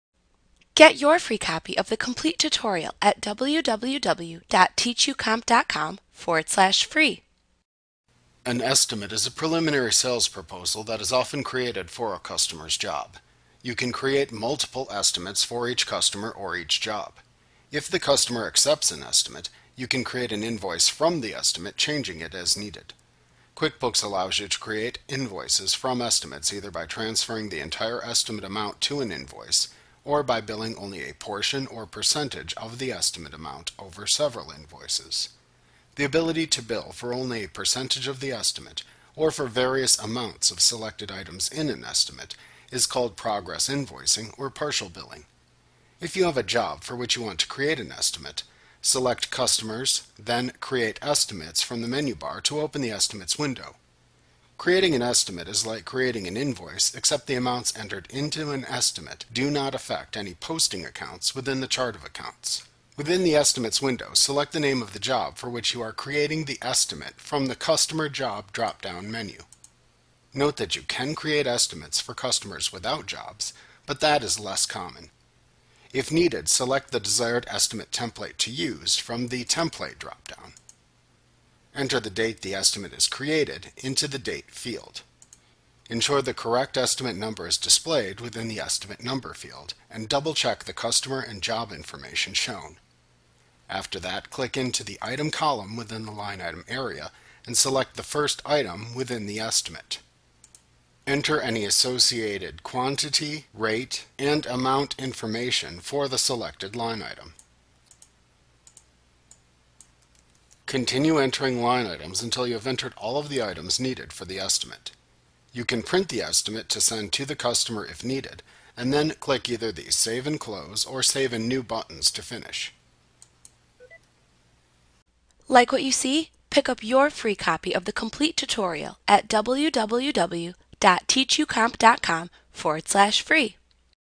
QuickBooks Pro 2017 Tutorial Creating an Estimate Intuit Training
Learn about Creating an Estimate in QuickBooks Pro 2017 A clip from Mastering QuickBooks Made Easy. - the most comprehensive QuickBooks tutorial available. Visit us today!